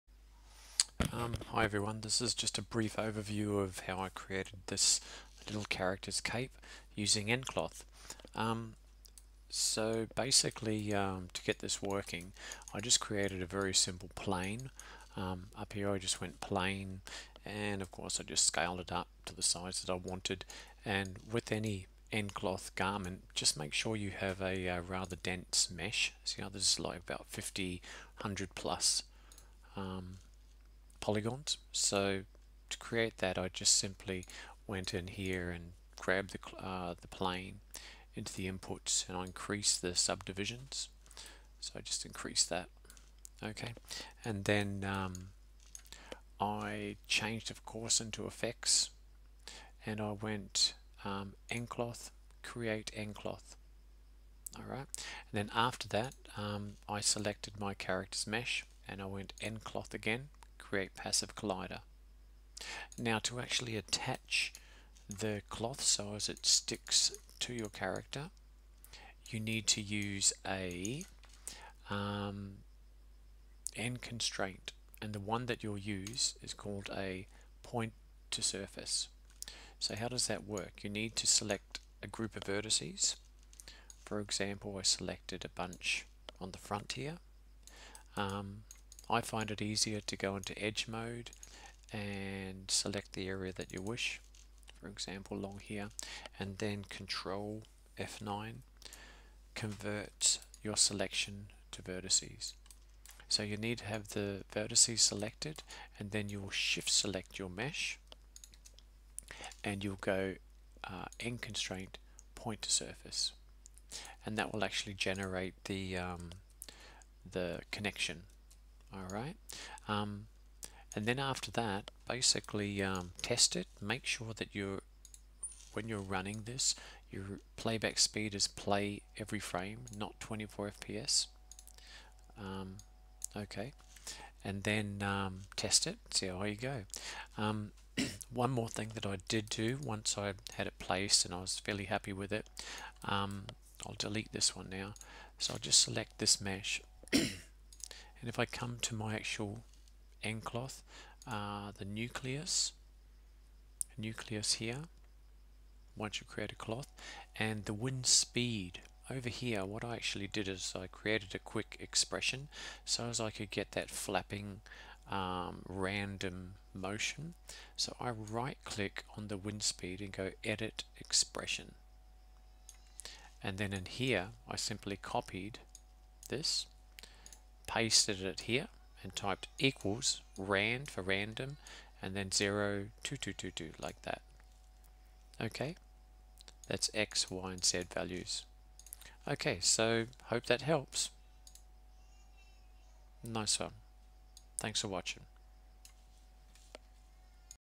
Maya nCloth Super Hero Cape Tutorial
Let's create an dynamic flowing super hero cape using nCloth in Maya 2020